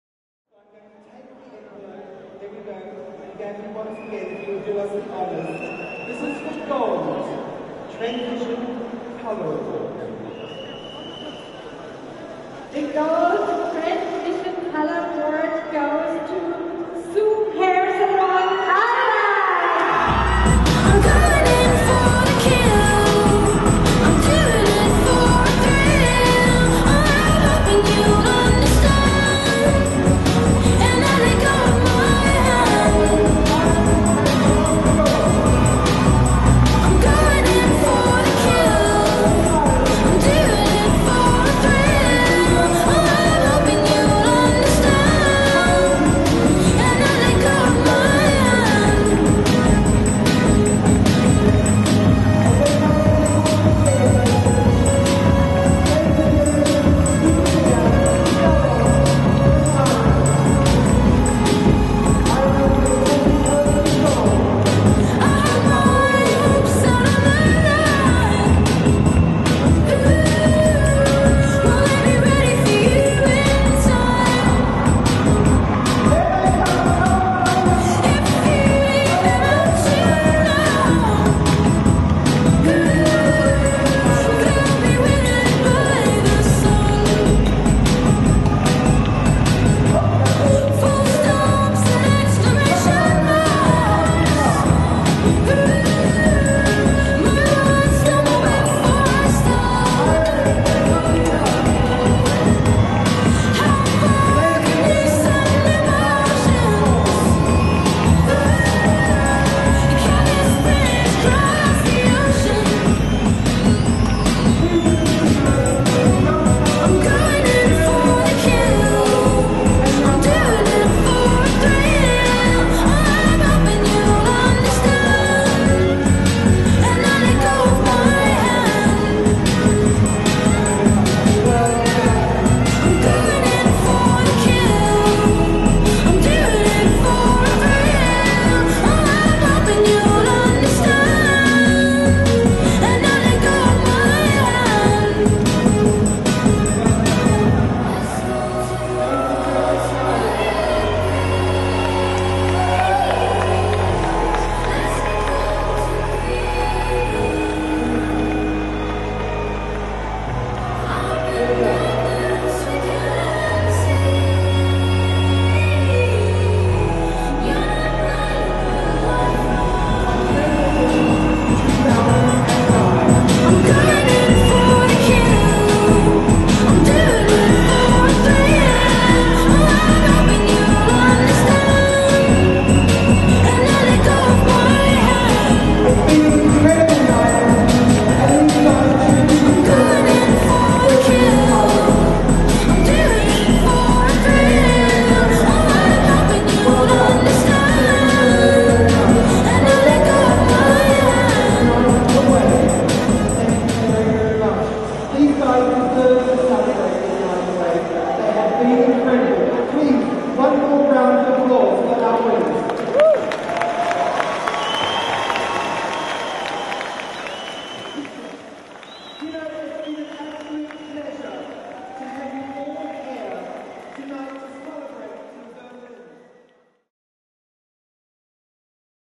Wella International Trend Vision Awards 2009 Berlin GOLD COLOR AWARD WINNER
Wella International Trend Vision Awards 2009 in Berlin, Germany. Andrew Dunne of Zoo Hair Salon in Ireland receiving the GOLD COLOR AWARD with model Kamila Moraczewska.